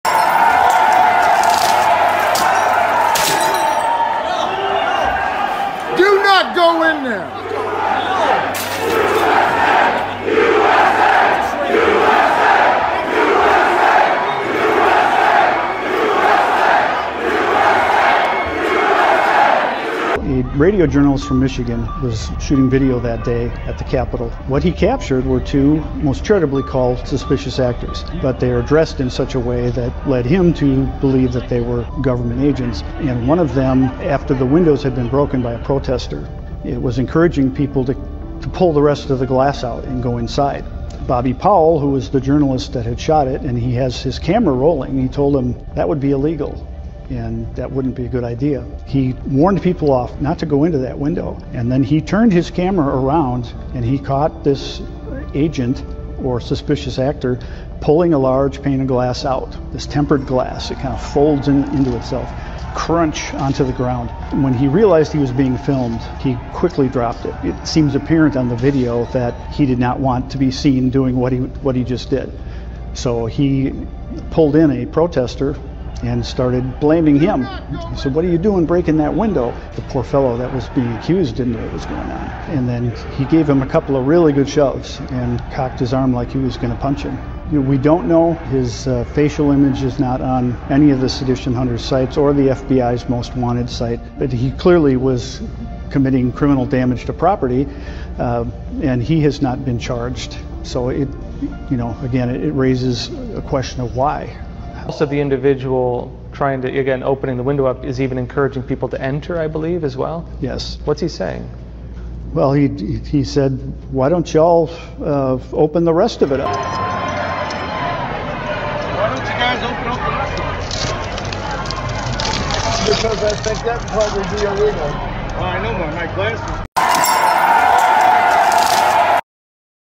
January 6 not an insurrection
As more and more videos come out, I mean we can say January 6 was never an insurrection begin with, but they’re going to continue push that agenda especially the one where it gets closer to election time. JoeBiden January6 destroyed DemocraticLiars ￼TakeBackWhatIsOurs